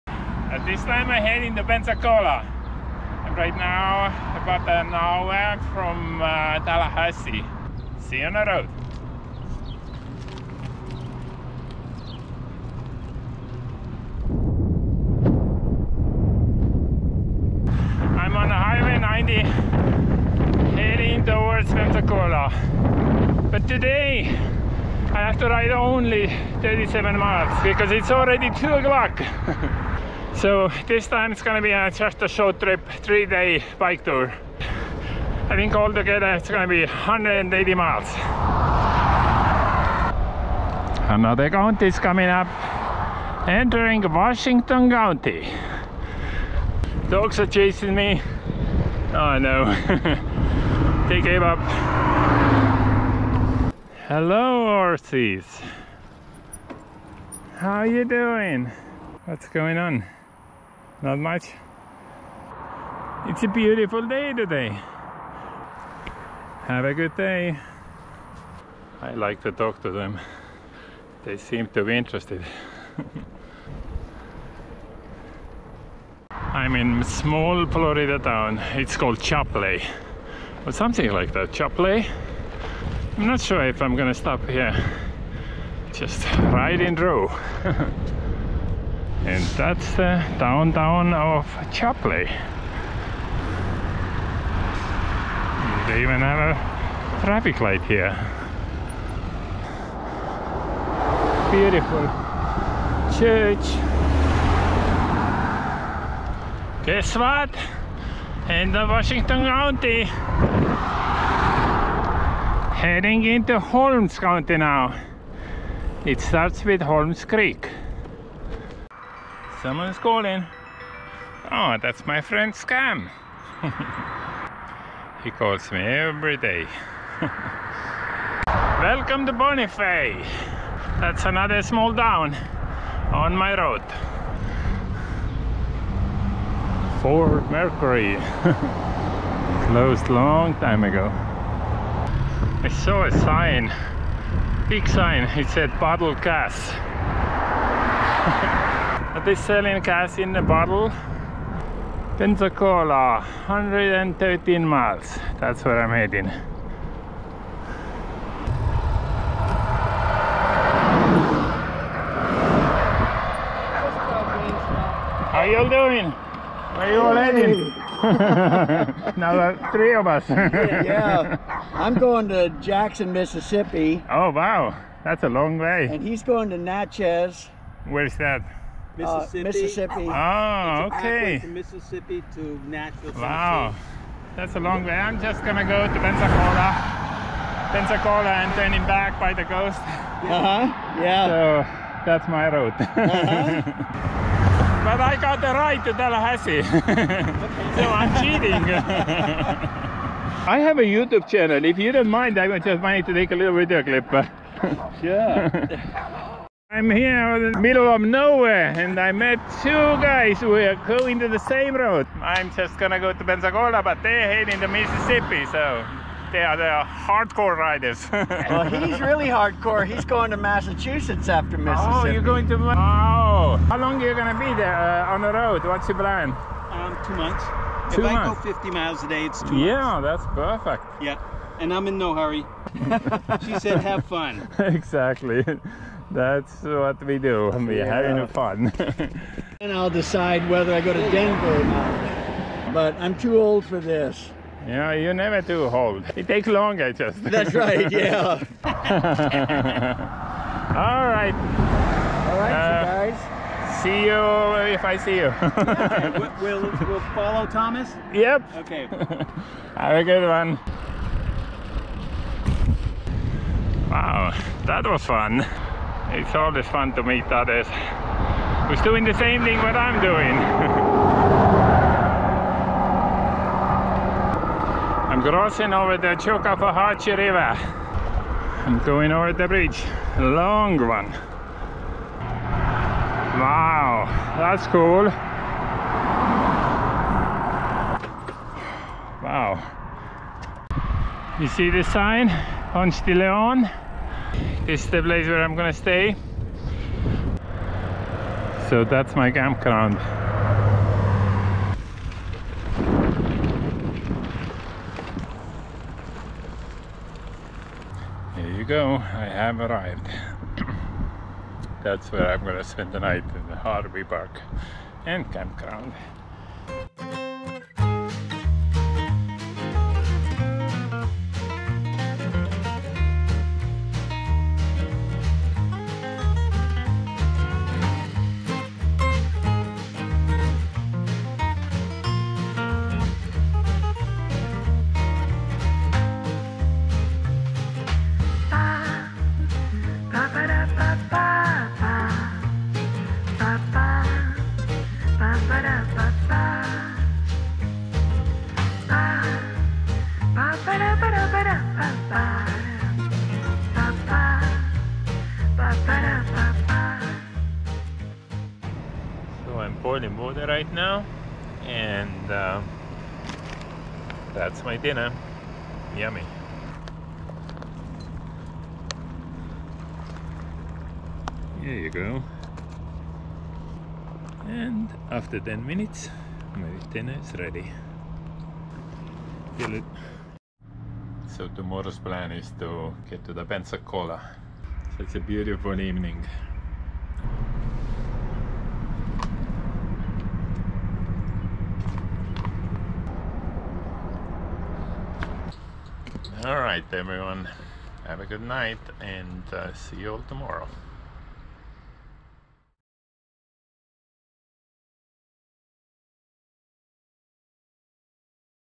DAY 1 - Pensacola Bike Trip. Riding alongside the Hwy 90 Meeting other bicycle travelers on the way
I'm at the Panhandle to ride about 200 miles, three days, from Tallahassee to Pensacola and finish my ride in Port Walton Beach. Florida has sixty-seven counties total. There are still seven counties left where I haven't ridden my bike and birded yet. This 3-day bike tour is planned to finish up all sixty-seven counties in Florida.